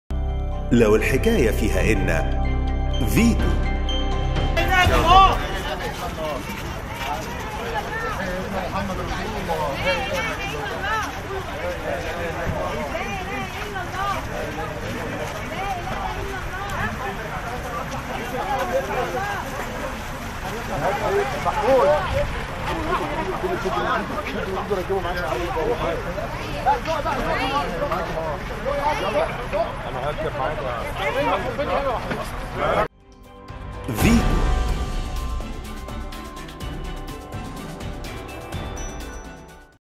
بوابة فيتو | خروج جثمان والد الفنان إيهاب توفيق من المستشفى بعد انتهاء تصاريح الدفن
خرج منذ قليل جثمان والد الفنان إيهاب توفيق من مستشفى التأمين الصحي بمدينة نصر، وذلك بعد انتهاء تصاريح الدفن،